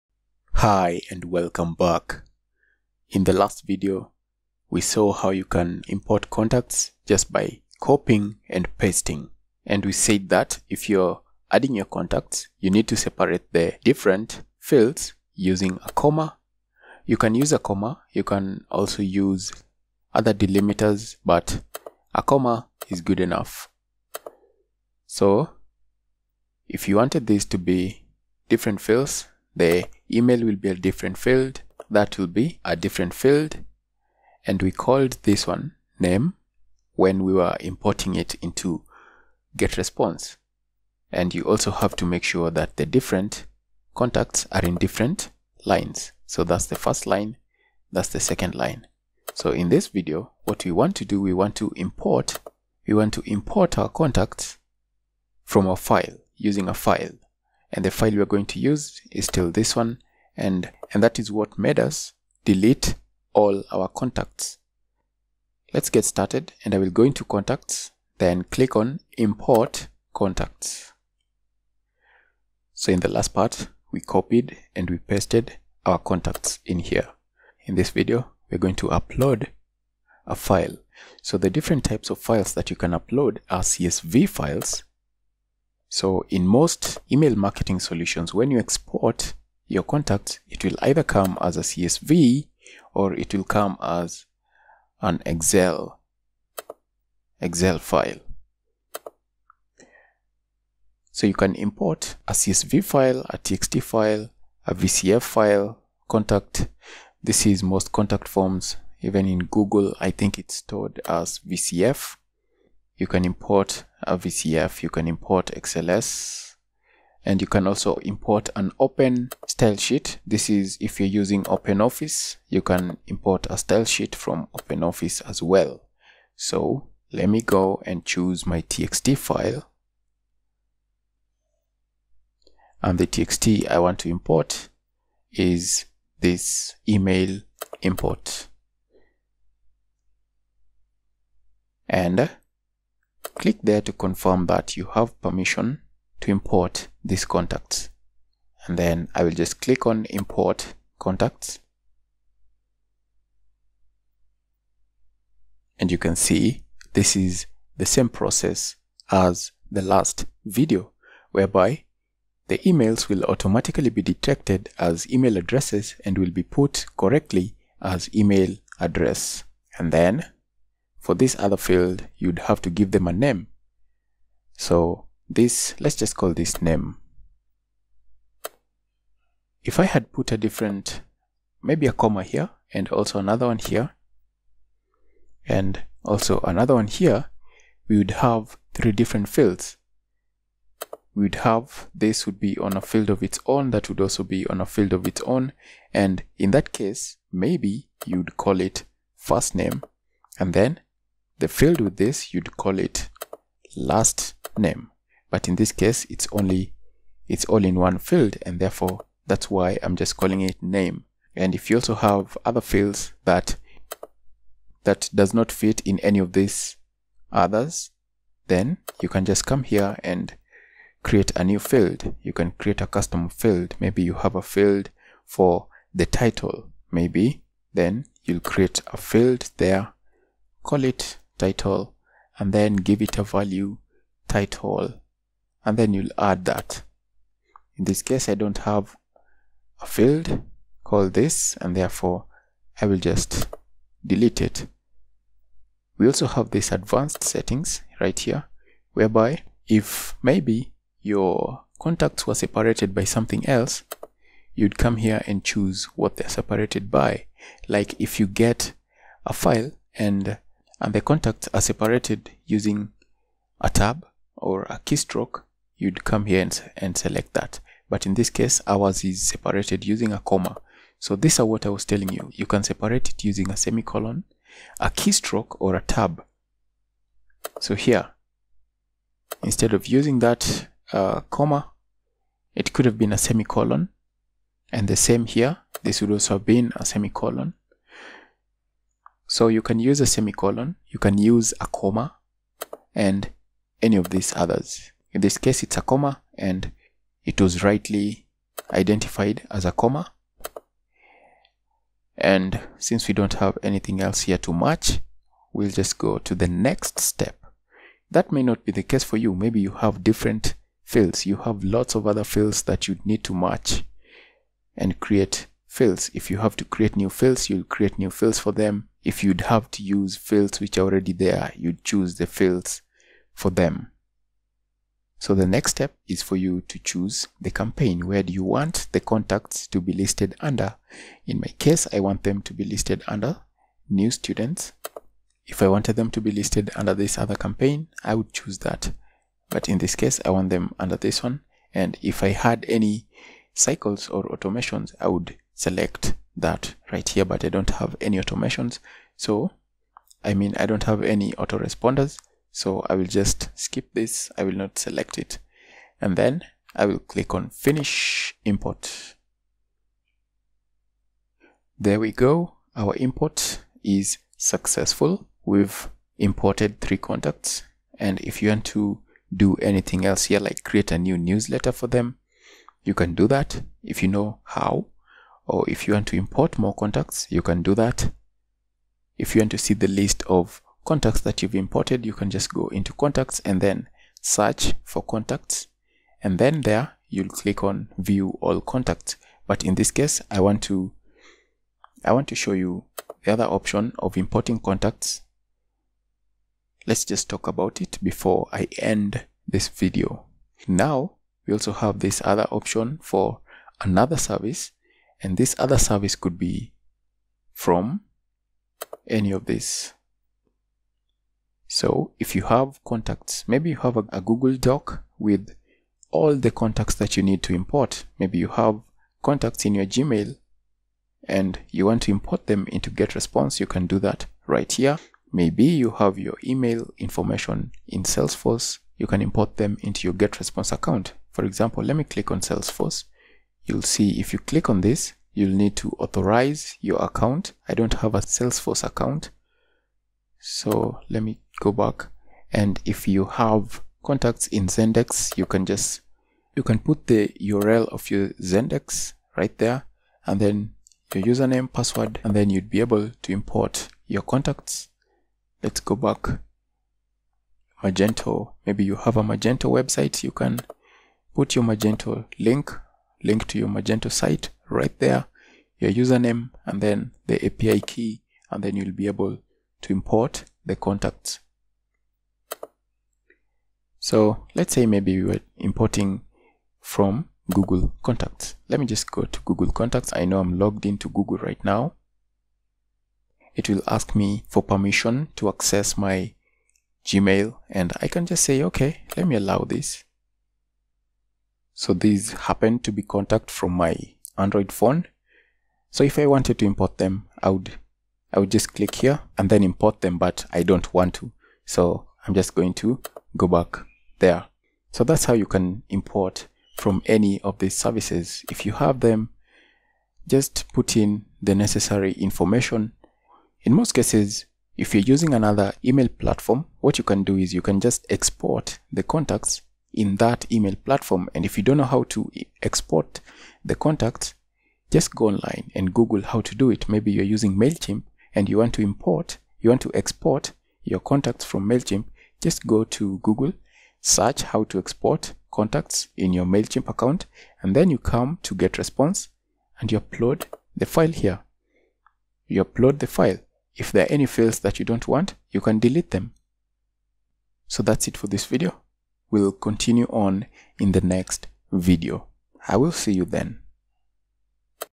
6 Import contacts into Getresponse as a file
6 Import contacts  into Getresponse as a file

getresponse tutorial,Getresponse,getresponse video tutorial, email marketing tutorial,email marketing

Hi, and welcome back.

In the last video, we saw how you can import contacts just by copying and pasting.

And we said that, if you’re adding your contacts, you need to separate the different fields using a comma.

You can use a comma, you can also use other delimiters.

But, a comma is good enough.

So, if you wanted this to be different fields, the email will be a different field, that will be a different field and we called this one Name when we were importing it into GetResponse.

And you also have to make sure that the different contacts are in different lines.

So that’s the first line, that’s the second line.

So in this video, what you want to do, you want to import, you want to import our contacts from a file, using a file.

And the file we’re going to use is still this one, and that is what matters.

Delete all our contacts.

Let’s get started and I will go into Contacts and click on Import Contacts.

So in the last part we copied and we pasted our contacts in here.

In this video, we’re going to upload a file.

So the different types of files that  you can upload are CSV files.

So in most email marketing solutions, when you export your contacts, it will either come as a CSV or it will come as an Excel, Excel file.

So you can import a CSV file, a TXT file, a VCF file contact.

This is most contact forms.

Even in Google I think it is stored as VCF.

You can import a VCF, you can import XLS, and you can also import an open stylesheet.

This is if you’re using OpenOffice.

You can import a stylesheet from OpenOffice as well.

So, let me go and choose my TXT file.

And the TXT I want to import is this email import.

And click there to confirm that you have permission to import these contacts.

And then I’ll just click on Import Contacts.

And you can see, this is the same process as the last video whereby the emails will automatically be detected as email addresses and will be put correctly as email address.

And then, for this other field, you’ll have to give them a name.

So, let’s just call this Name.

If I had put a different, maybe a comma here, and also another one here, and also another one here, we’d have 3 different fields.

We’d have, this would be on a field on it’s on, that would also be on a field of it’s on.

And in that case, maybe you’d call it First Name.

And then, the field with this, you’d call it Last Name.

But in this case, it’s all in one field and therefore, that’s why I’m just calling it name.

And if you also have other fields that doesn’t fit in any of this, others, then you can just come here and create a new field.

You can create a custom field.

Maybe you have a field for the title.

Maybe then you create a field there.

Call it Title, and then give it a value, Title.

And then you’ll add that.

In this case, I don’t have a field called this and therefore I will just delete it.

We also have this advanced settings right here, whereby if maybe your contacts were separated by something else, you’d come here and choose what they’re separated by.

Like if you get a file, and the contacts are separated using a tab, or a keystroke, you’d come here and select that.

But in this case, ours is separated using a comma.

So this is what I was telling you.

You can separate it using a semi-colon, a keystroke or a tab.

So here, instead of using that comma, it could have been a semi-colon and the same here, this would also have been a semi-colon.

So you can use a semi-colon, you can use a comma and any of these others.

In this case it’s a comma and it was rightly identified as a comma and since we don’t have anything else here too much, we’ll just go to the next step.

That may not be the case for you.

Maybe you have different fields.

You have lots of other fields that you need to match, and create fields.

If you have to create new fields, you’ll create new fields for them.

If you’d have to use fields which are already there, you choose the fields for them.

So the next step is for you to choose the campaign.

Where do you want the contacts to be listed under.

In my case, I want them to be listed under New Students.

If I want them to be listed under the other campaigns, I would choose them.

But in this case, I want them under this one.

And if I had any cycles or automations, I would select that right here.

But I don’t have any automations.

So, I mean I don’t have any autoresponders.

So I will just skip this.

I will not select it.

And then I will click on Finish Import.

Our import is successful.

We’ve imported 3 contacts.

And if you want to do anything else here like create a new newsletter for them, you can do that if you know how.